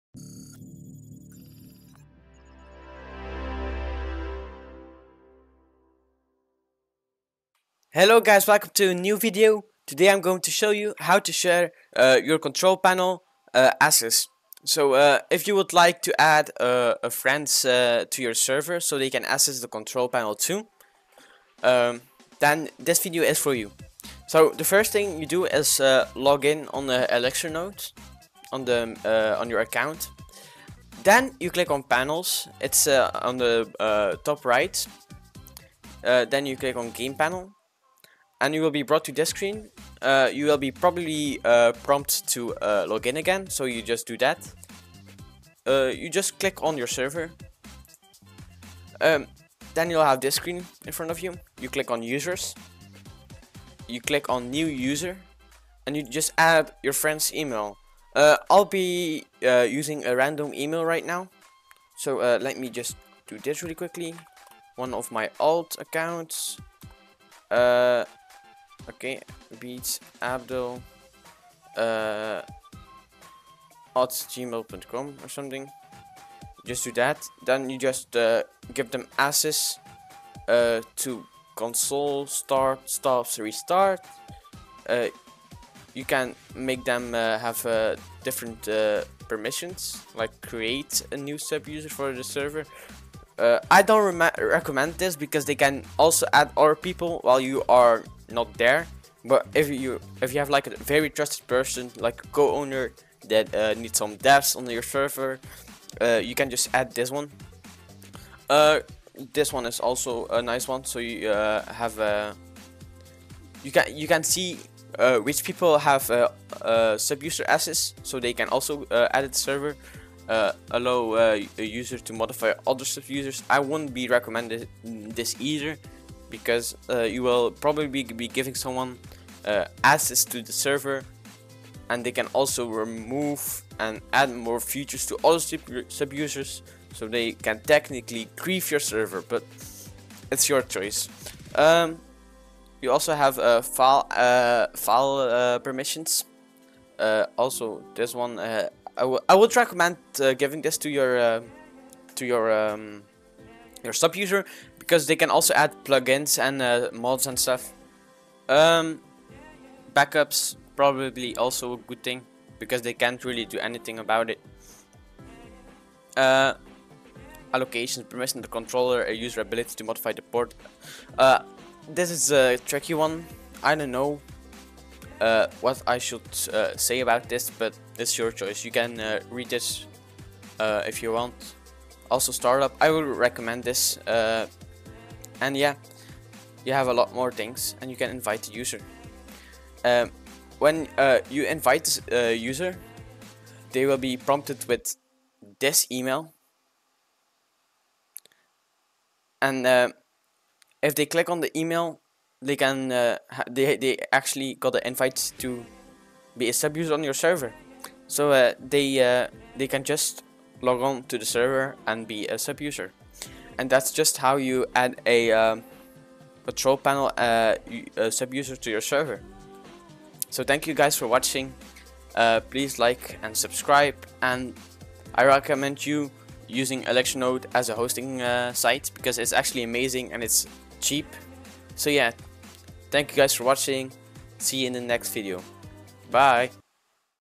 How share control panel access to your game server
In this video, we will show how to share control panel access to your Game server (such as Minecraft server).